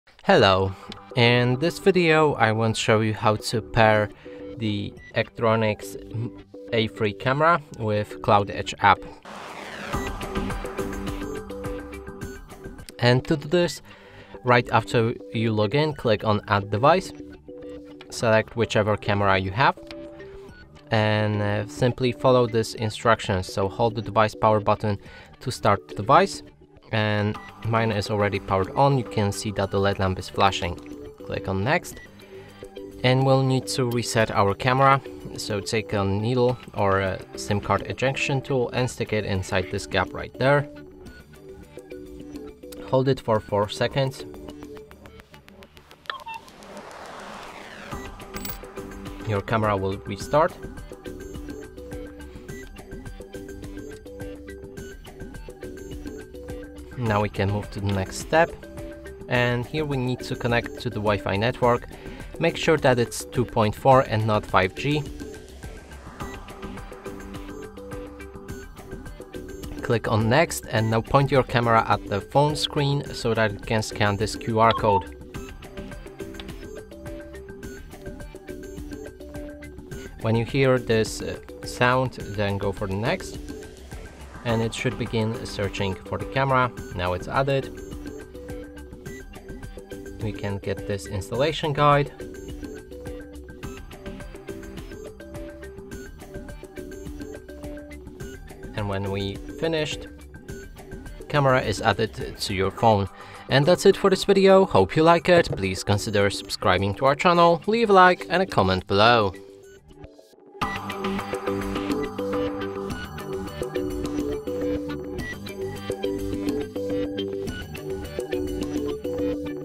How To Pair Ctronics A3 With App
In this video tutorial, we will guide you through the process of pairing the Ctronics A3 camera with the accompanying mobile app. The Ctronics A3 camera offers advanced features and functionality that can be accessed and controlled through its dedicated app. We will show you step-by-step how to download and install the app on your smartphone or tablet.